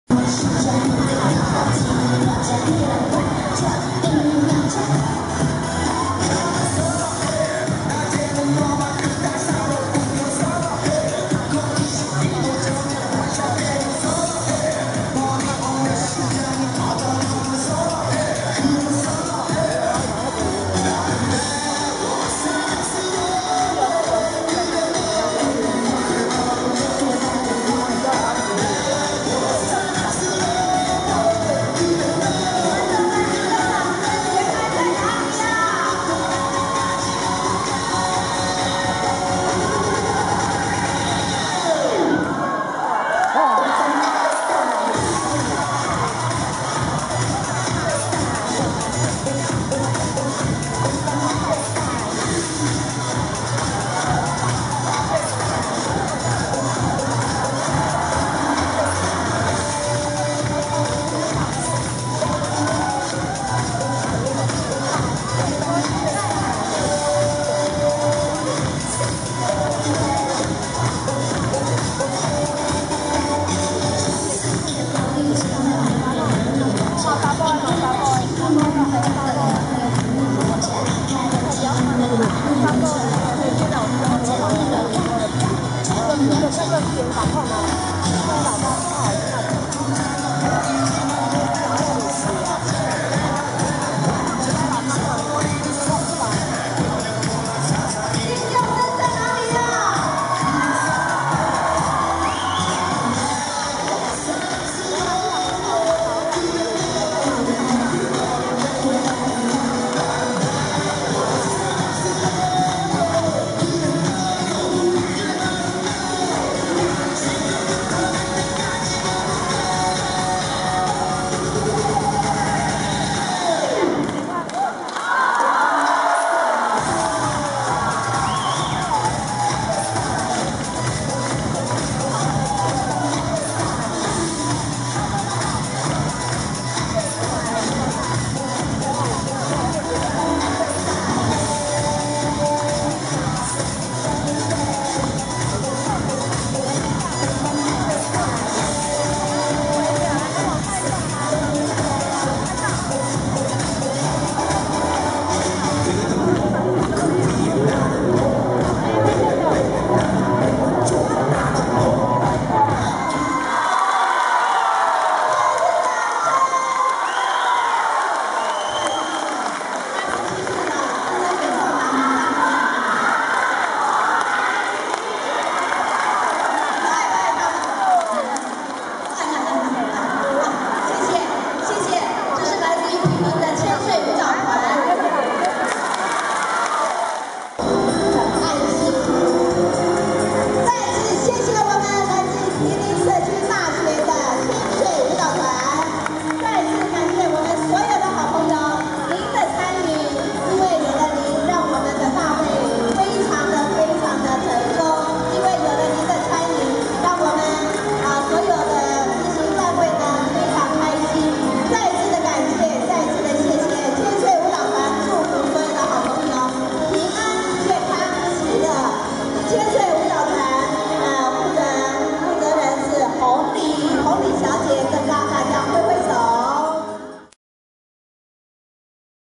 活力無限的千歲舞蹈團
新北市政府衛生局主辦的「長者健康活力SHOW」，由年齡加起來超過一千歲的爺爺奶奶們組成的「千歲舞蹈團」，勇奪金牌。